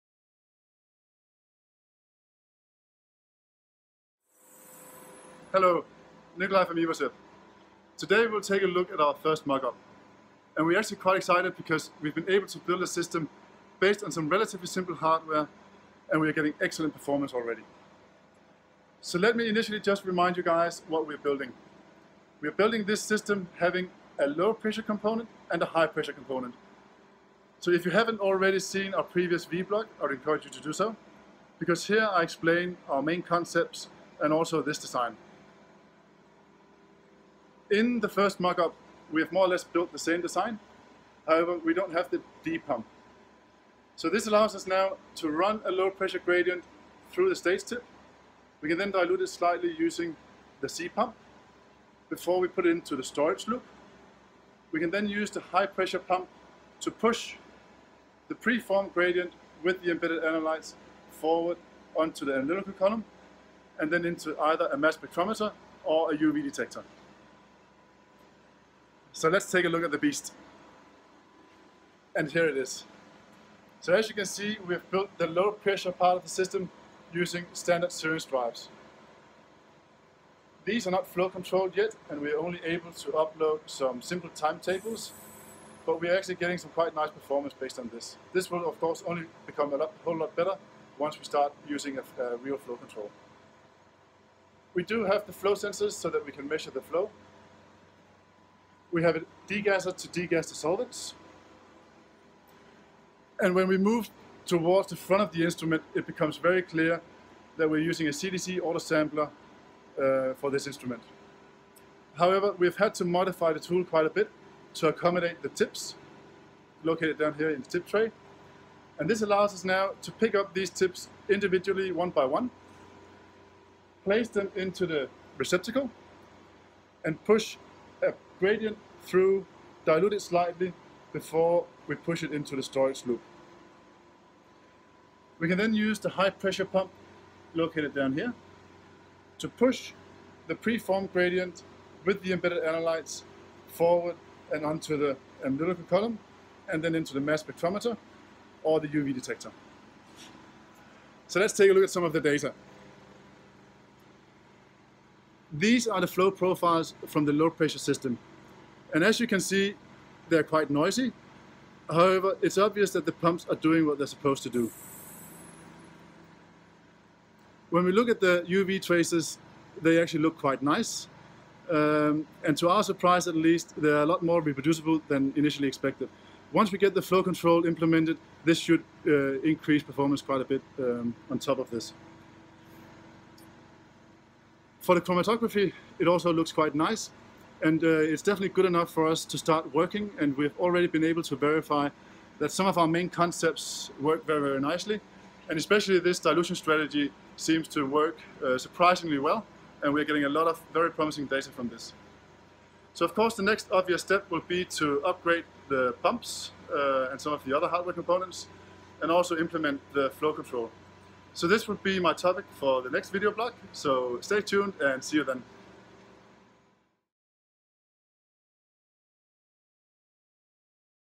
The first simple mockup
Nicolai Bache explains the technical details of our very first and simple prototype.